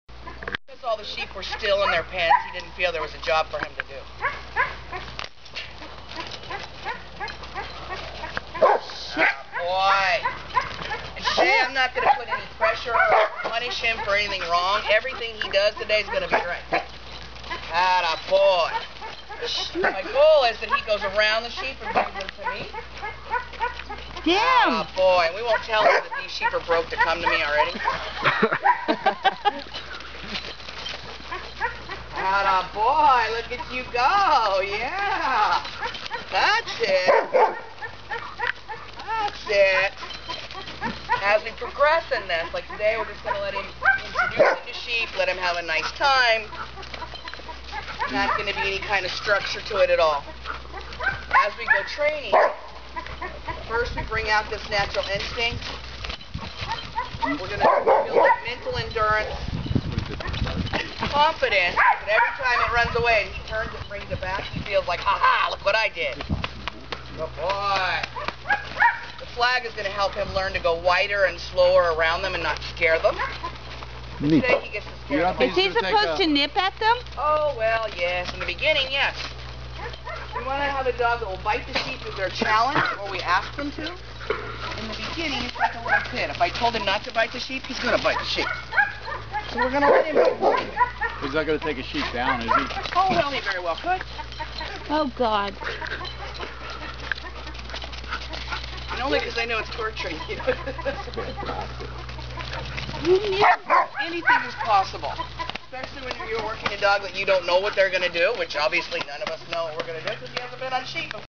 Bowie starts to herd
Bowie went a herding today! When we told Robin that we thought he was part border collie, she said she didn't think so...then she put him in the pen with the sheep. Bowie RULED! She said only two breeds have the ability to 'eye' the sheep like he did, and they are border collies and kelpies. She now thinks he is border collie and bearded collie mix.